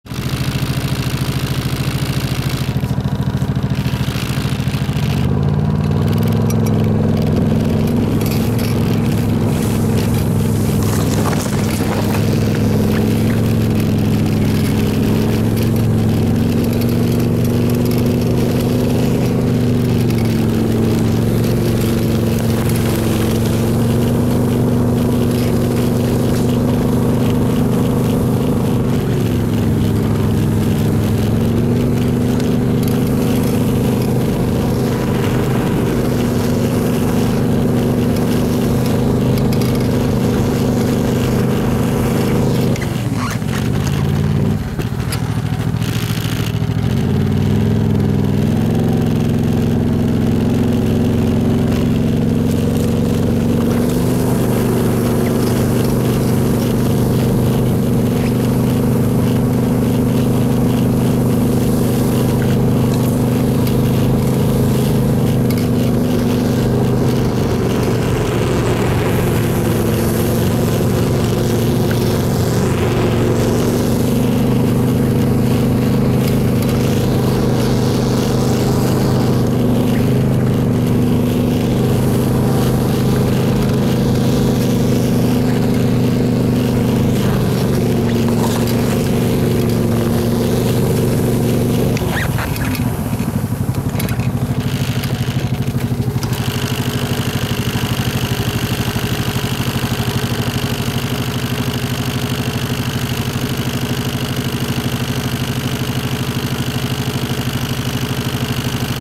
27 12 16 В целик на мототолкаче, в глубокий снег.
Проезд по путикам, на мототолкаче. Глубокий рыхлый снег, неслежавшийся. Показана проходимость толкача.